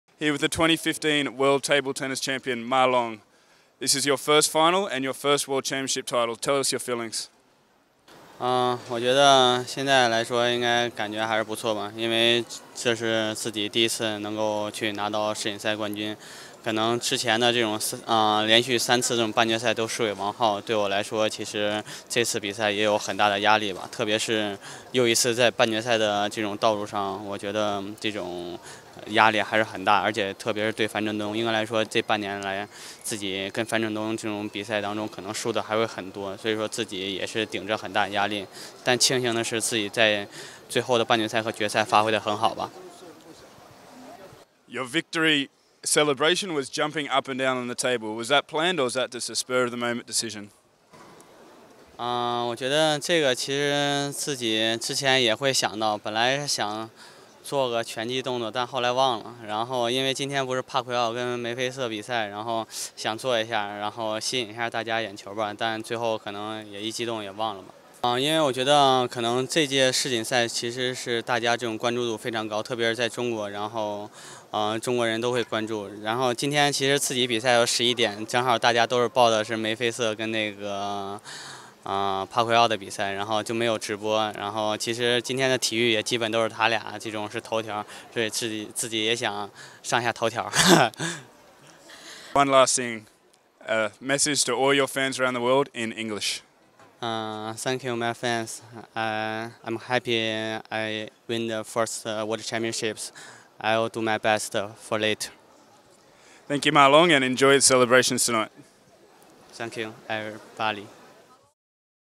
Ma Long - Qoros 2015 World Championships Interview
Find out what the newly crowned 2015 Men's Champion Ma Long has to share on his victory at the Qoros 2015 World Championships!